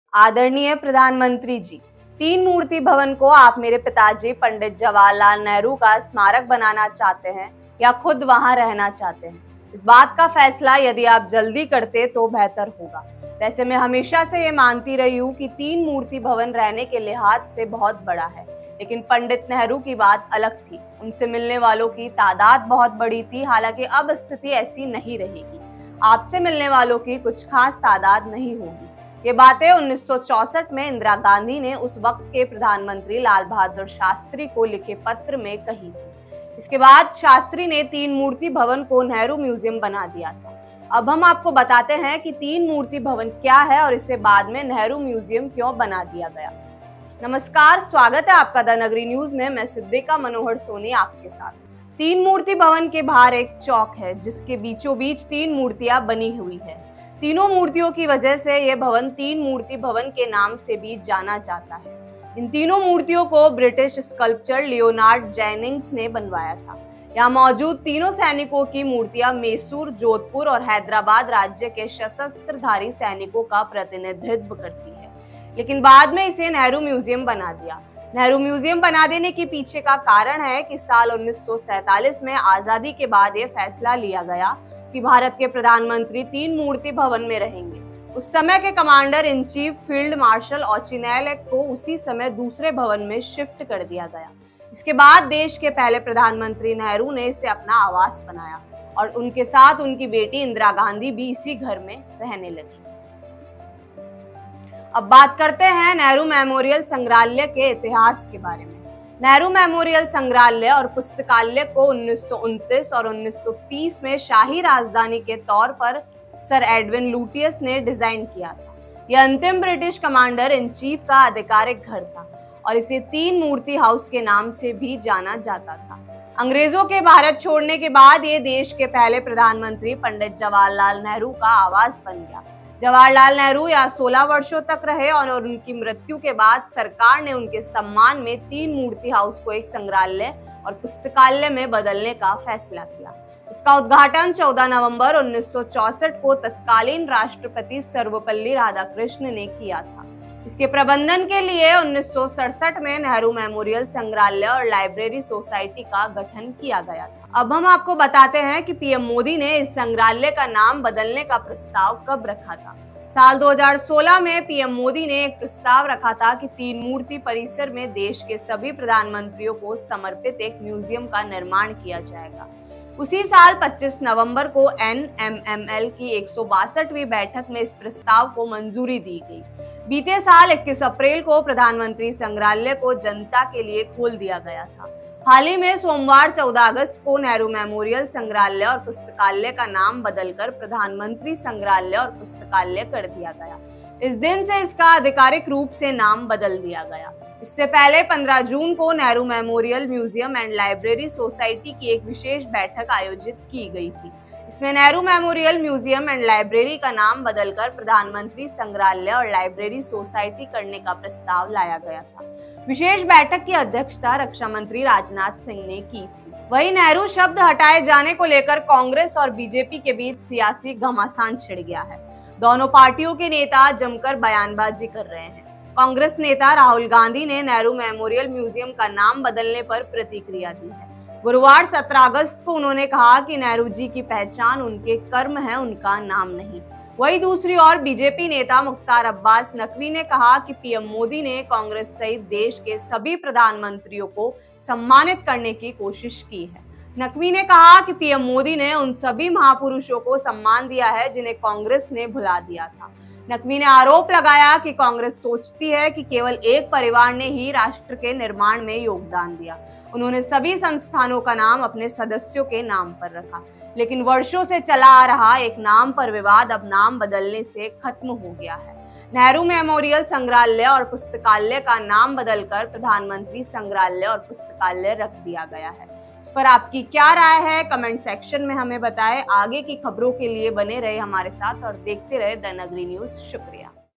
नेहरू मेमोरियल संग्रहालय अब हुआ प्रधानमंत्री संग्रहालय || Narendra Modi || Jawahar Lal Nehru
नेहरू मेमोरियल संग्रहालय अब हुआ प्रधानमंत्री संग्रहालय || Narenddra Modi || Jawahar Lal Nehru